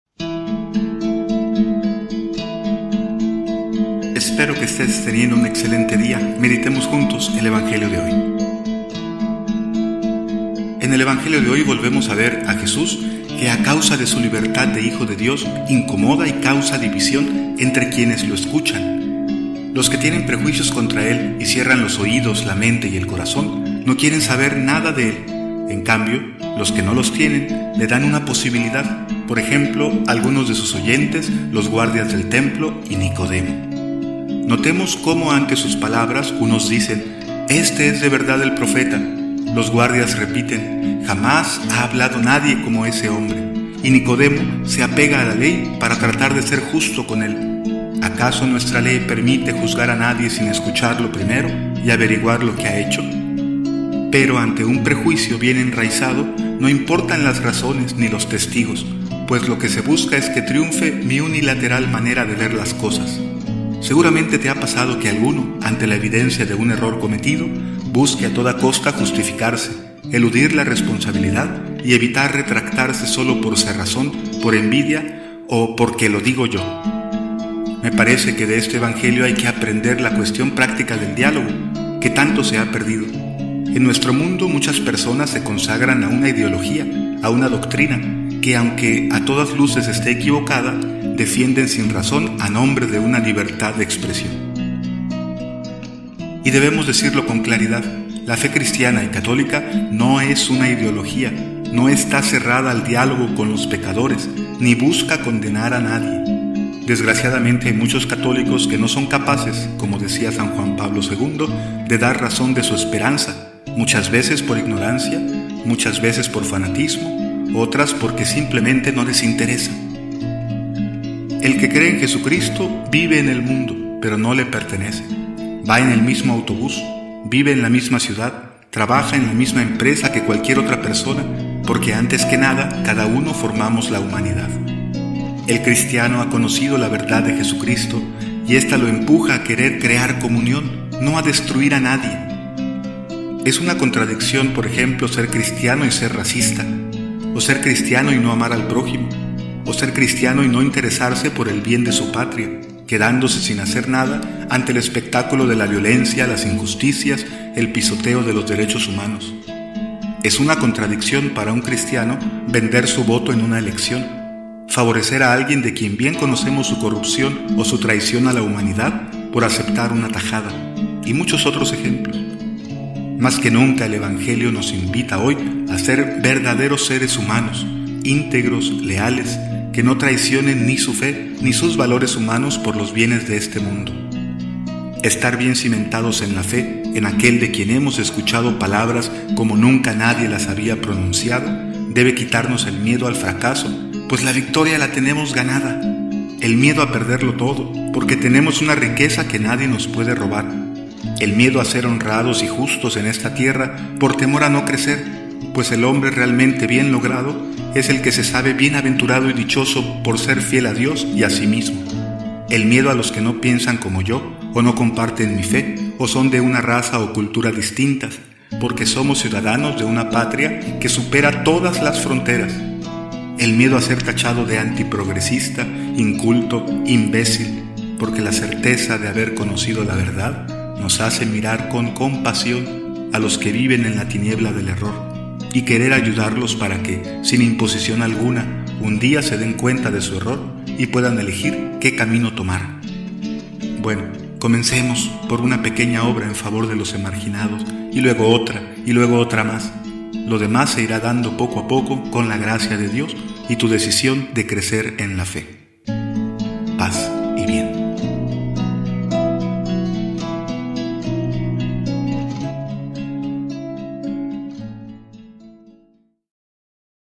"Open Mind" - Evangelio del 17/03/2018 - Sábado IV de Cuaresma - Jn 7, 40-53 (hecho con Spreaker)
Te invito a visitar mi blog y compartirlo con tus amigos

¡Sígueme!

¡CoMPaRTe!

fray alex
Evangelio Día a Día
17/03/2018